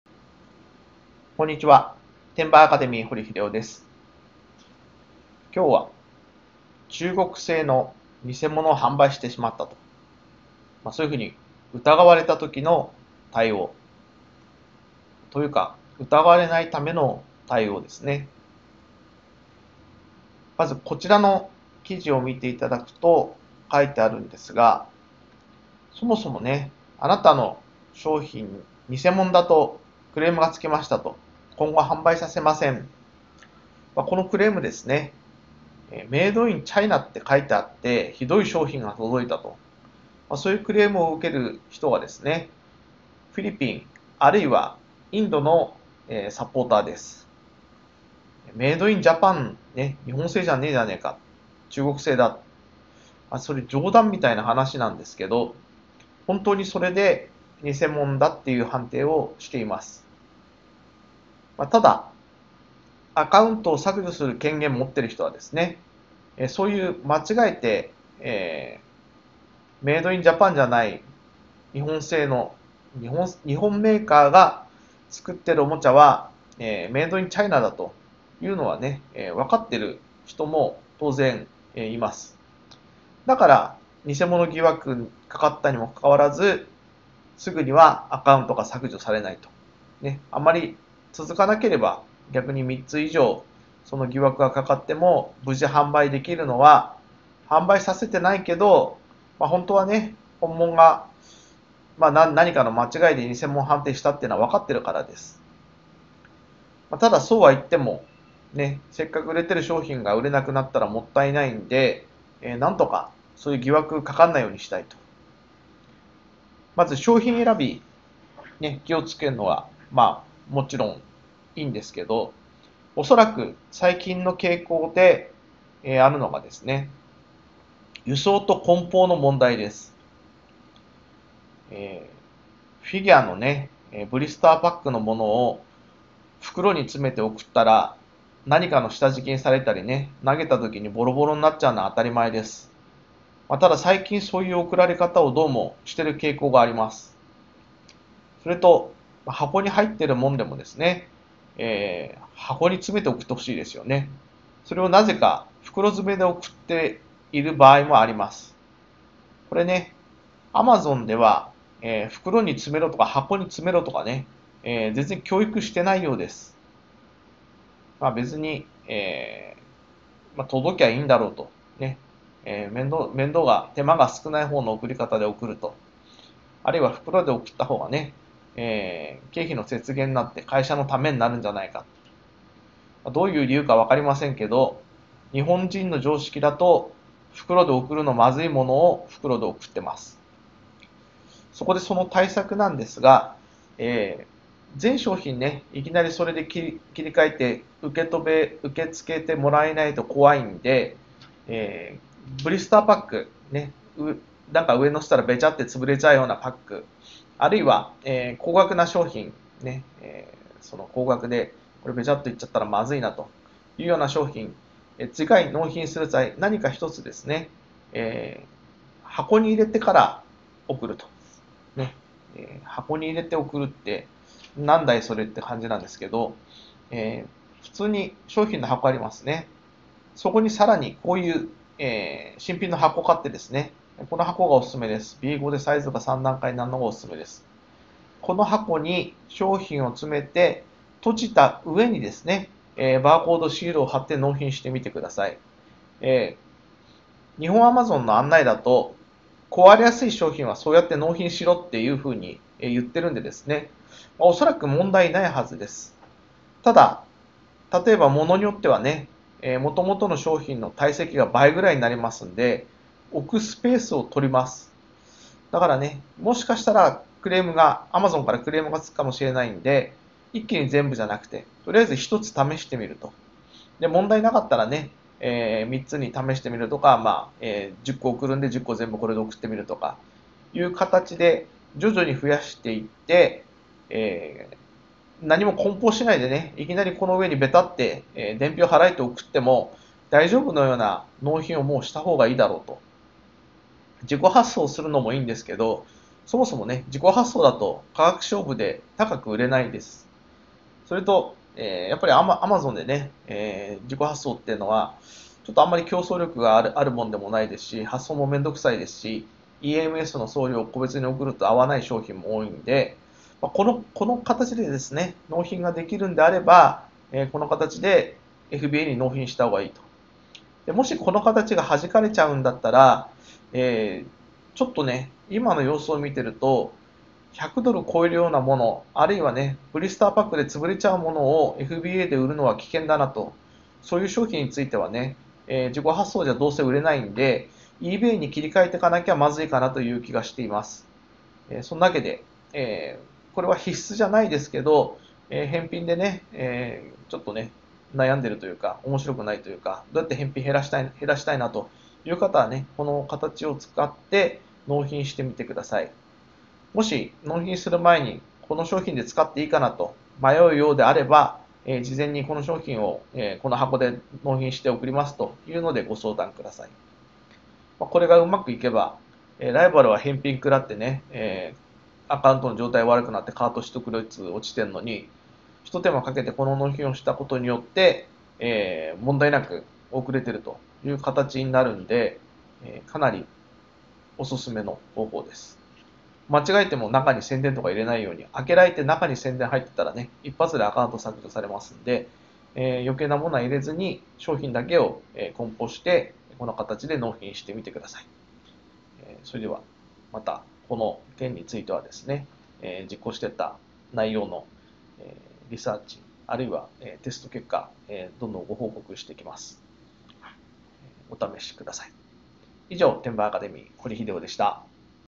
アマゾンでいいがかりの偽物疑惑をかけられないようにする対策
2016年03月09日に会員限定で公開した動画です。

1日10分で月10万円を稼ぐ! Amazon輸出ビジネスがわかる本　出版社:ソシム (2013/11/22)